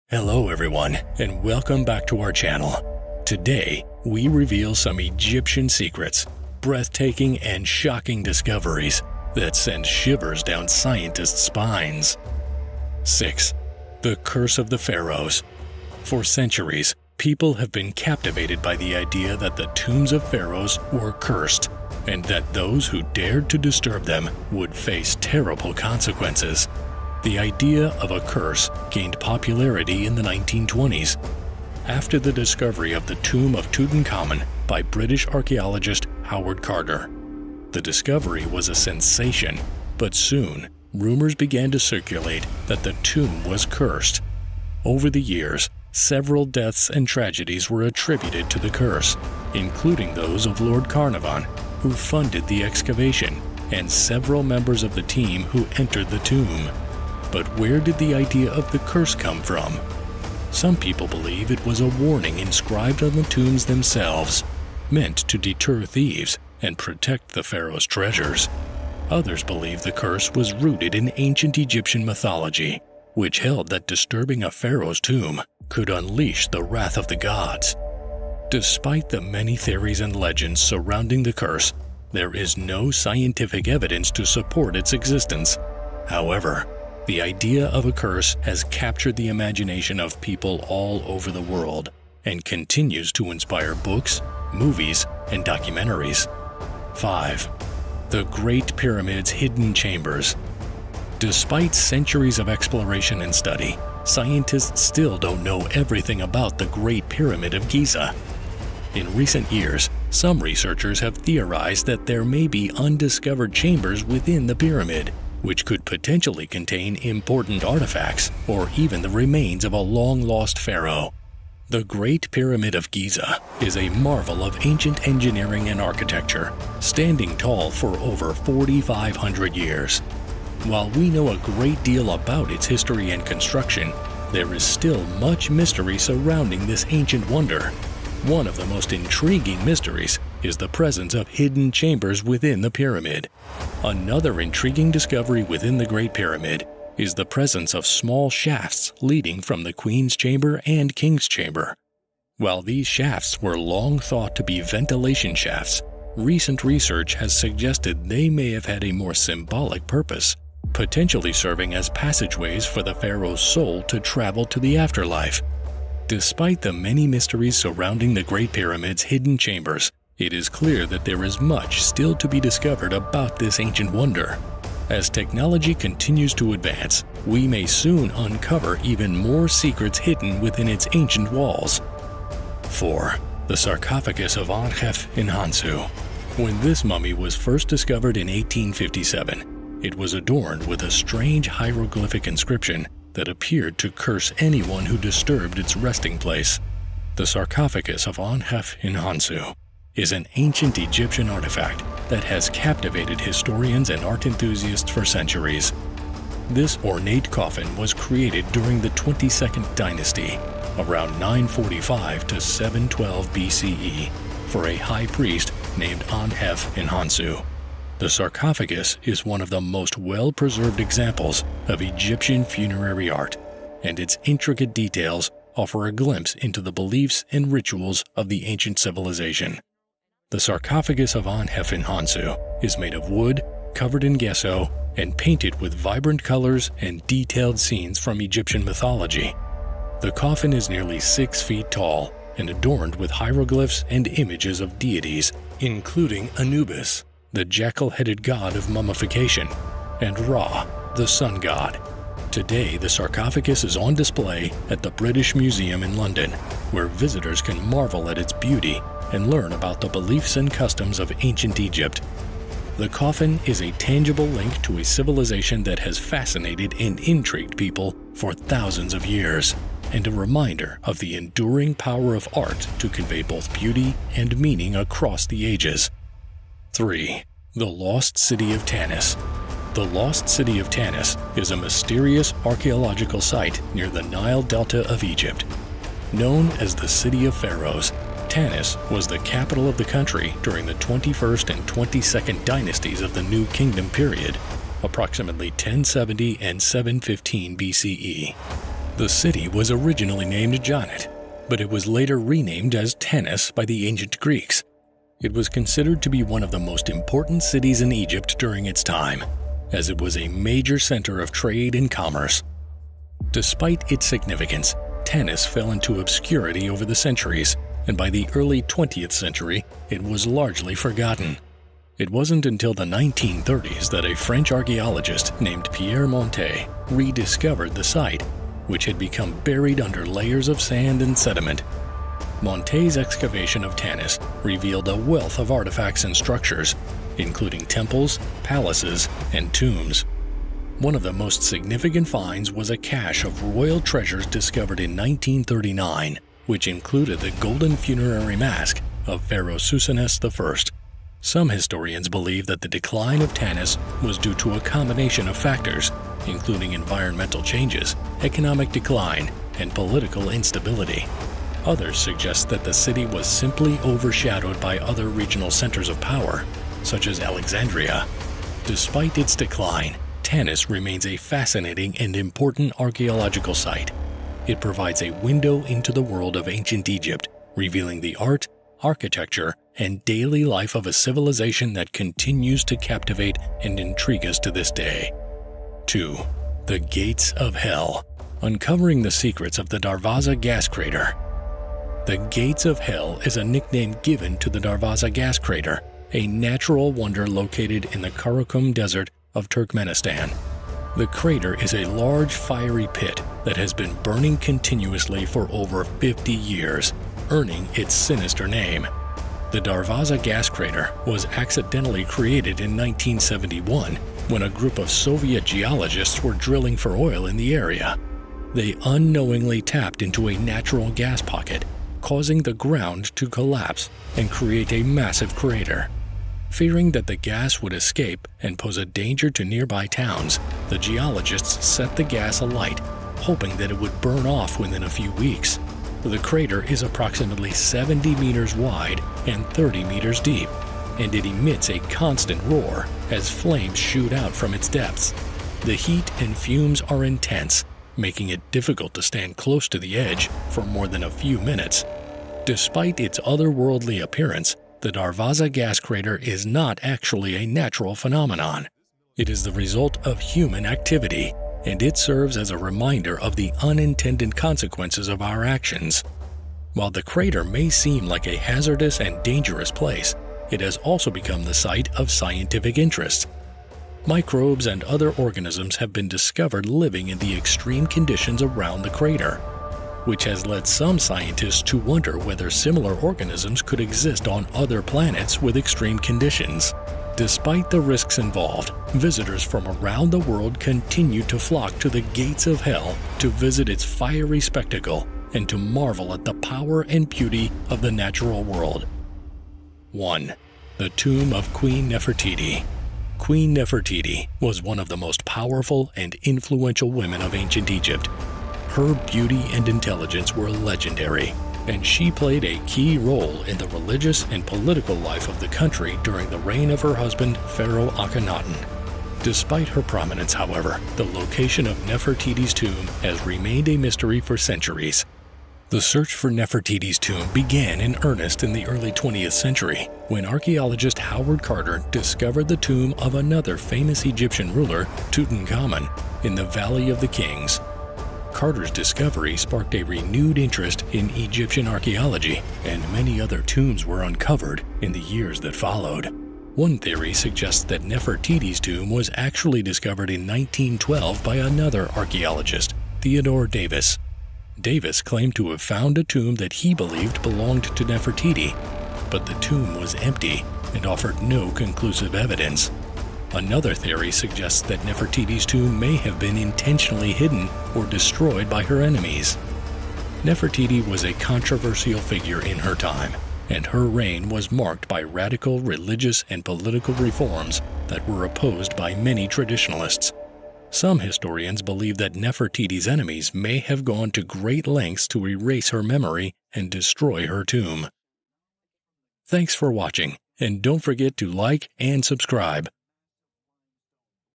6 BREATHTAKING and SHOCKING Discoveries in Egypt That TERRIFY Scientists - Part 1
Today, we reveal some Egyptian secrets. Breathtaking and Shocking Discoveries That Send Shivers Down Scientists spines.

6. The Curse of the Pharaohs
5. The Great Pyramid's Hidden Chambers
4. The Sarcophagus of Ankh-ef-en-Khonsu
3. The Lost City of Tanis
2. The "Gates of Hell"
1. The Tomb of Queen Nefertiti

About History Viral
🎥 Videos about History
🎨 Written, voiced and produced by History Viral